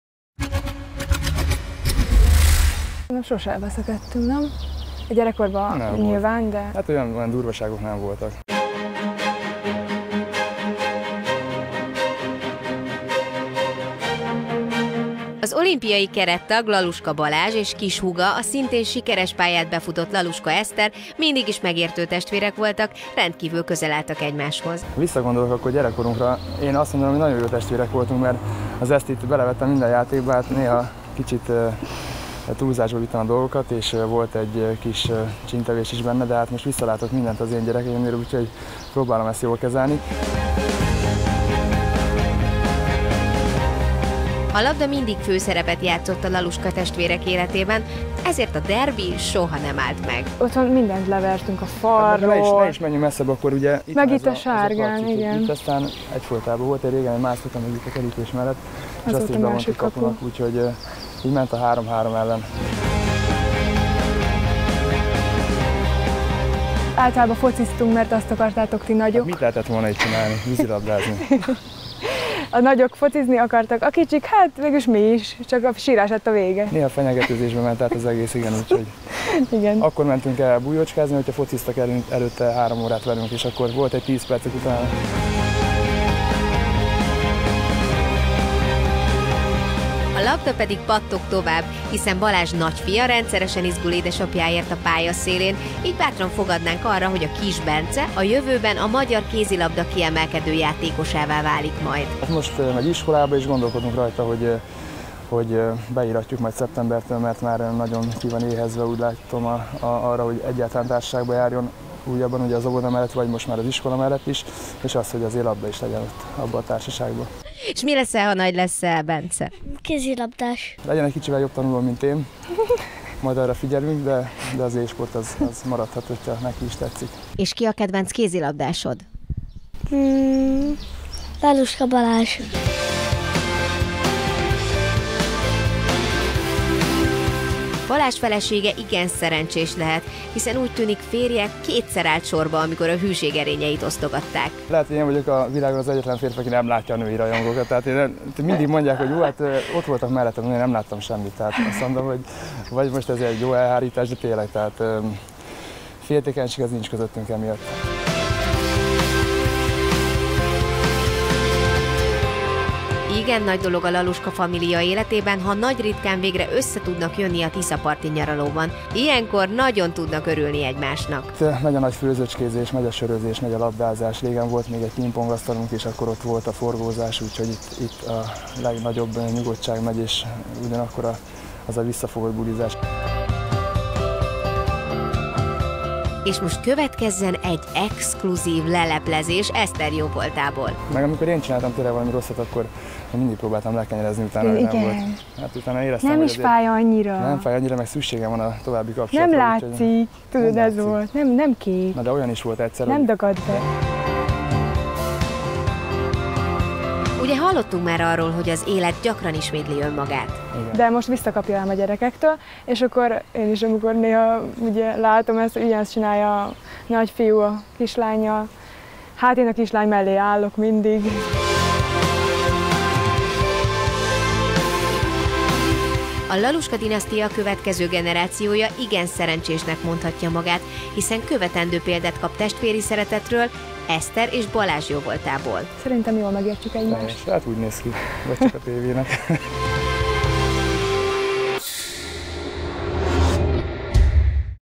Laluskáéknál mindent visz a kézilabda
Az MKB Veszprémben szereplő Laluska Balázs igazi sportos családban él. Húga, Eszter szintén kézilabdázott, és úgy tűnik, hogy tovább gyarapodik a kézilabdás dinasztia.